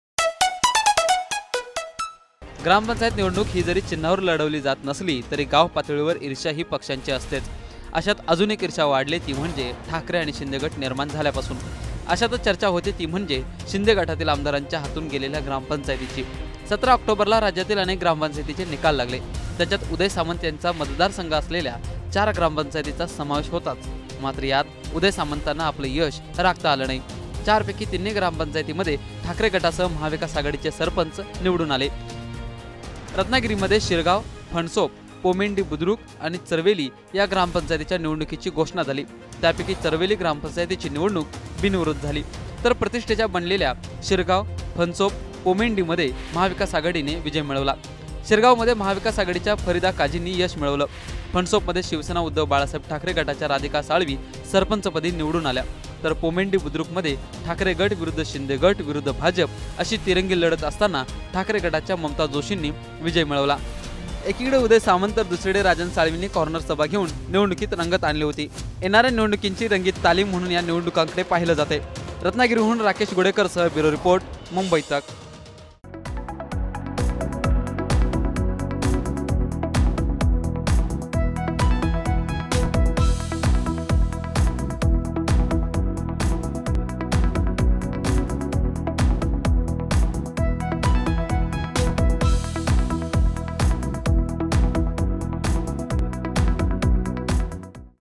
Uday Samant यांचा त्यांच्याच मैदानात पराभव, Uddhav Thackeray यांनी मारली बाजी | Rajan Salvi
17 ऑक्टोबरला ग्रामपंचायत निवडणुकीचा निकाल लागला, यावेळी उदय सामंत यांना रत्नागिरी मतदारसंघामध्ये एकही ग्रामपंचायत राखता आली नाही.